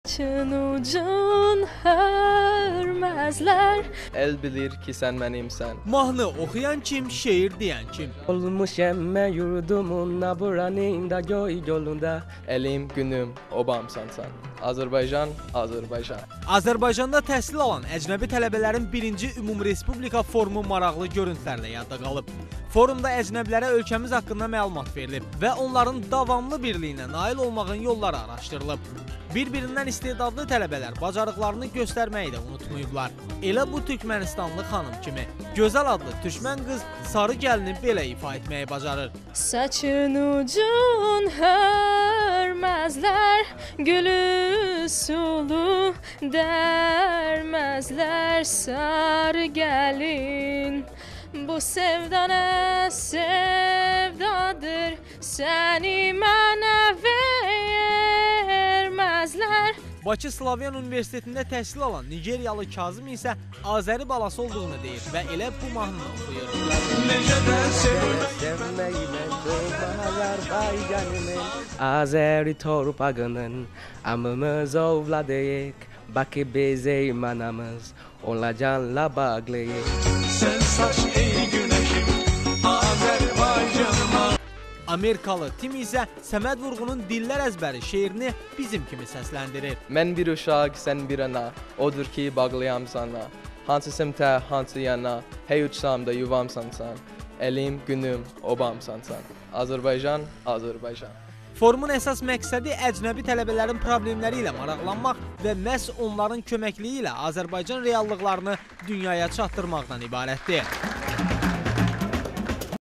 Forum of Foreign Students in Azerbaijan - Xazar TV
Azərbaycanda təhsil alan xarici tələbələrin I ümumrespublika Forumu - Xəzər TV reportajı